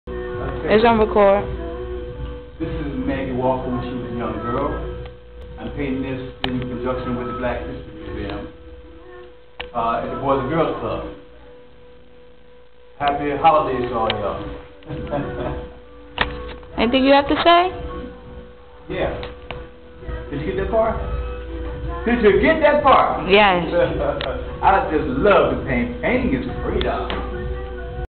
I love to paint
Sir James L. Thornhill 2010  painting Maggie L. Walker in her younger days. This mural project was in conjunction with the Black History Museum and Cultural Center of Richmond Va